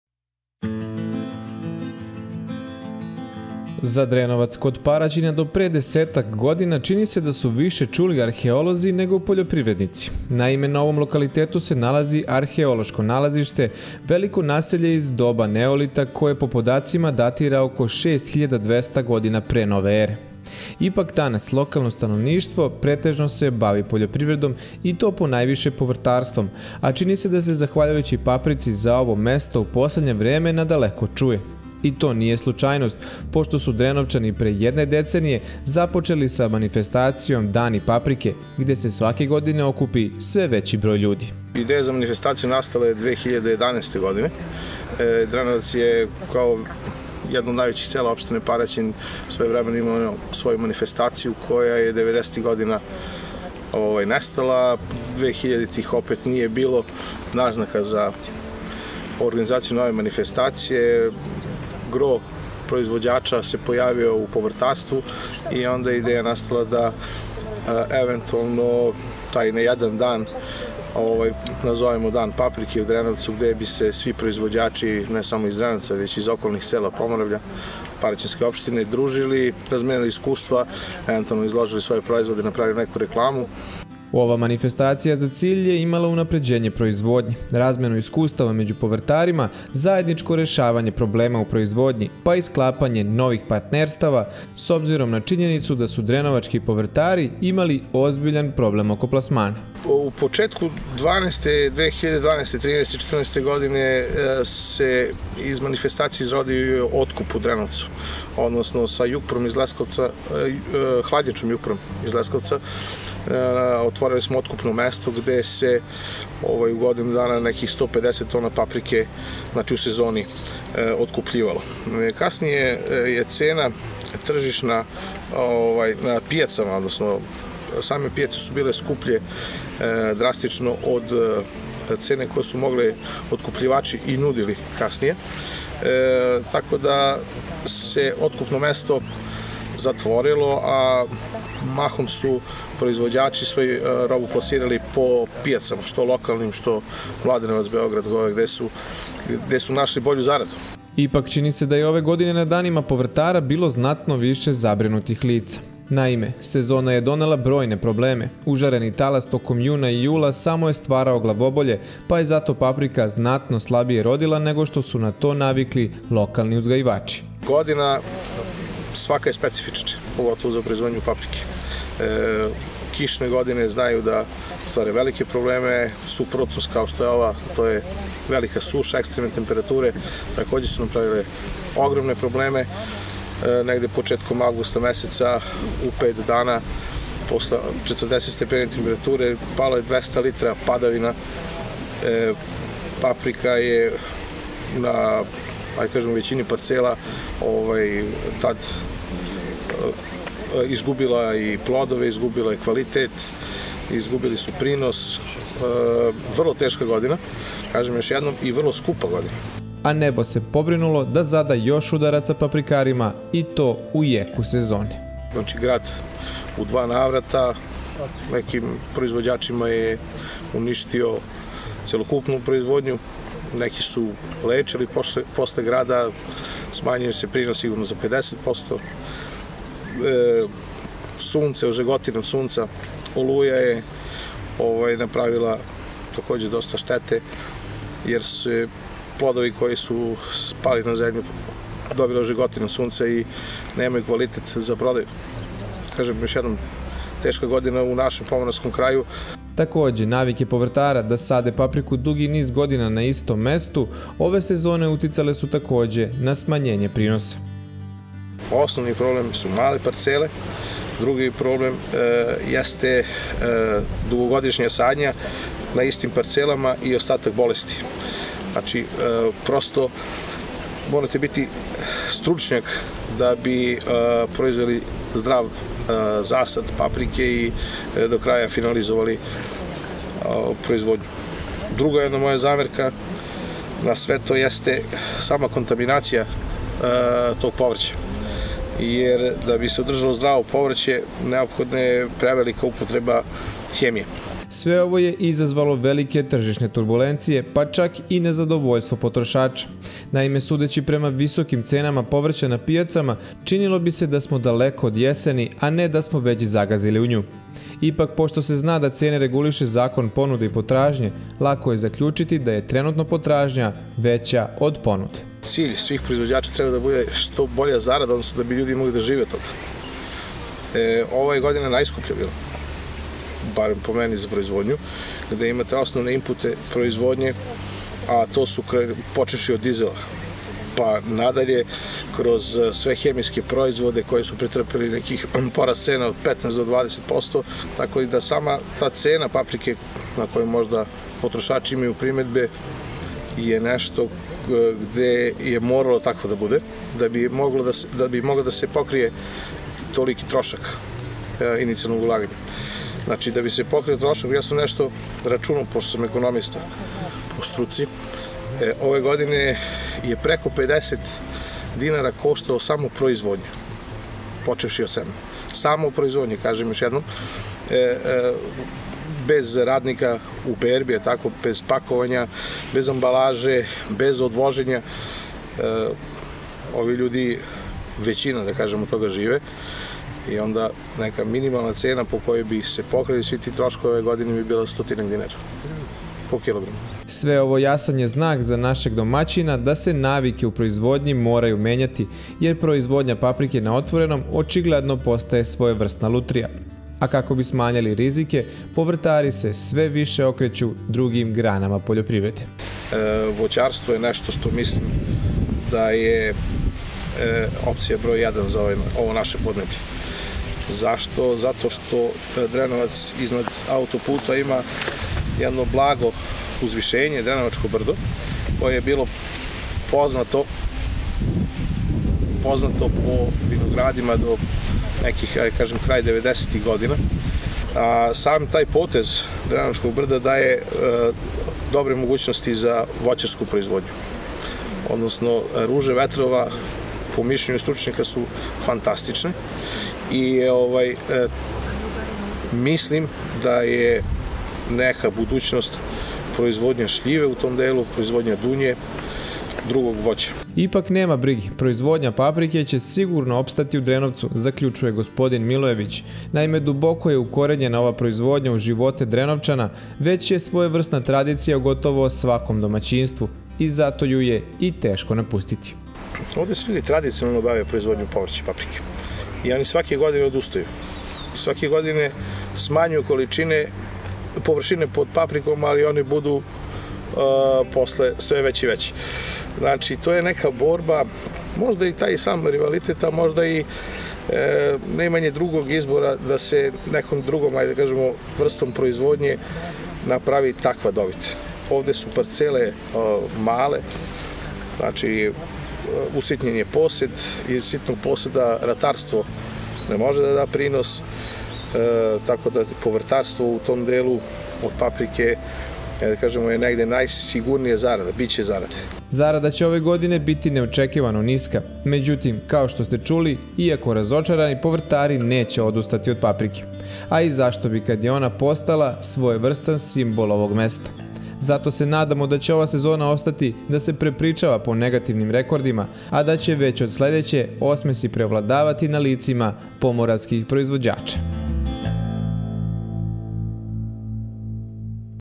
TEŠKI DANI za domaće paprikare - Drenovac
Sa kakvim izazovima su se ove sezone suočili paprikari iz Drenovca?
Sagovornik: Goran Milojević - Drenovac
Facebook : Farma emisija o poljoprivredi